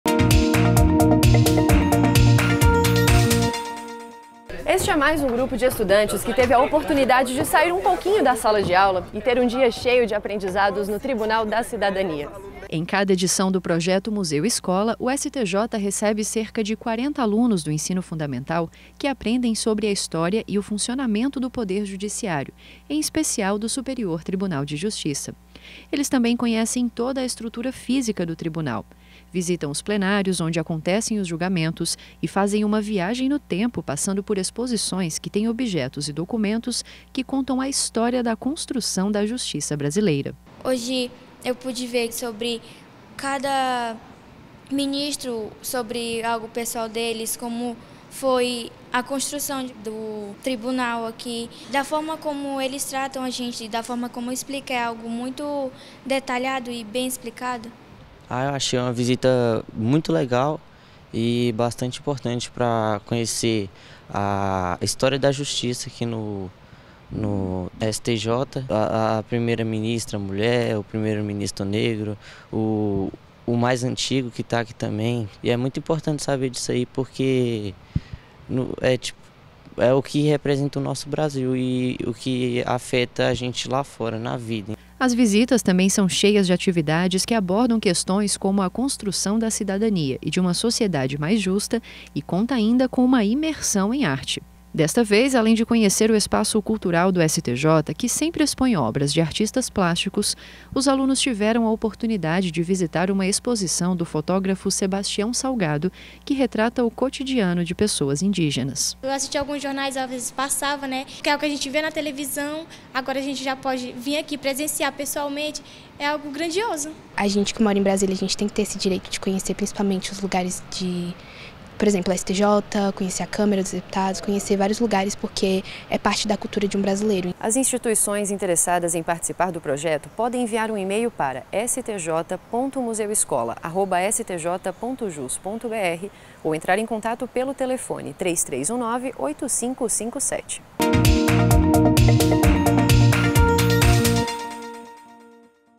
Projeto Museu-Escola volta a receber visita de estudantes do ensino fundamental
Cerca de 40 alunas da escola Vila das Crianças, de Santa Maria – DF, visitaram o Tribunal, nessa terça (2), em mais uma edição do projeto Museu-Escola, que retoma suas atividades neste semestre. As estudantes do 8º ano do ensino fundamental chegaram de manhã e foram conduzidas ao mezanino dos plenários pela equipe da Seção Educativa e Social (SEDUC/SED).